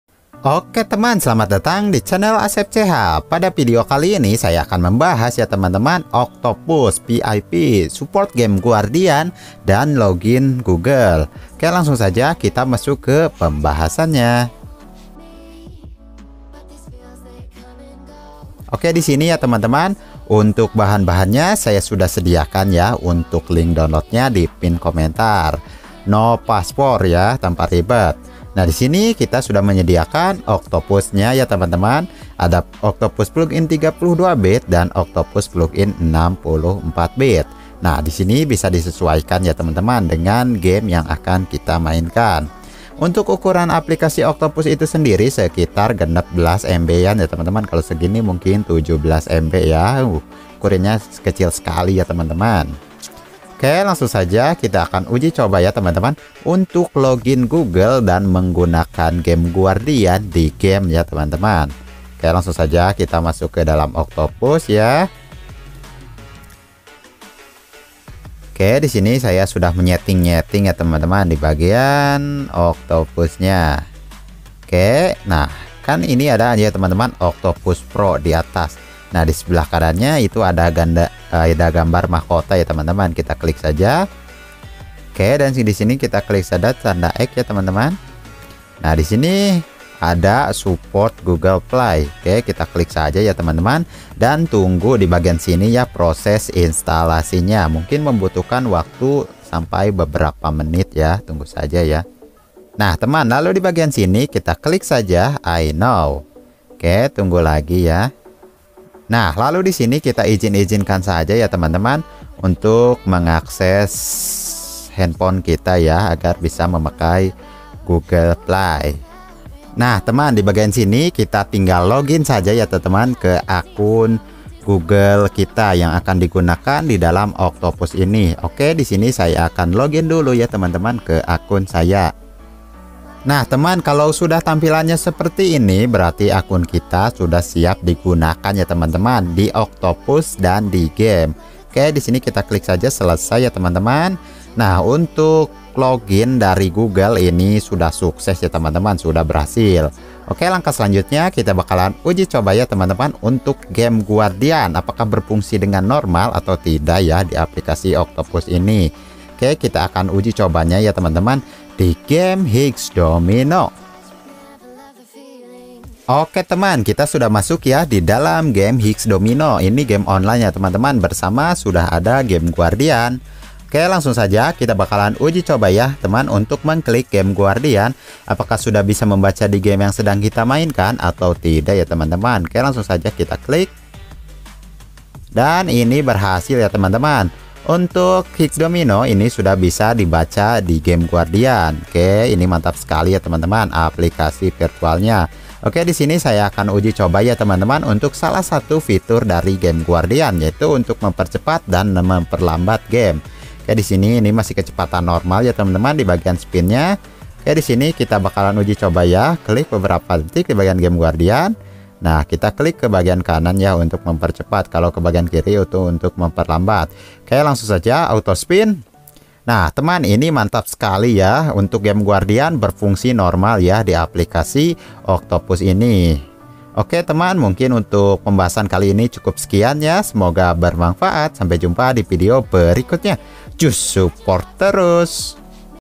Octopus Support Game Guardian + Support Login Google - Apakah Bisa?
Octopus VIP Support Game Guardian + Support Login Google

Pada video kali ini saya akan membahas sebuah aplikasi virtual Octopus, yang dimana Octopus ini sudah VIP didalamnya.
Aplikasi Octopus ini sudah bisa login Google, dan juga sudah bisa menggunakan Game Guardian tanpa root.

Octopus adalah aplikasi ruang virtual yang memungkinkan penggunanya untuk bisa menambahkan aplikasi dan game yang diinginkan kedalamnya.
Aplikasi Octopus ini support keyboard, mouse, gamepad, dan juga sudah bisa pasang Game Guardian tanpa root.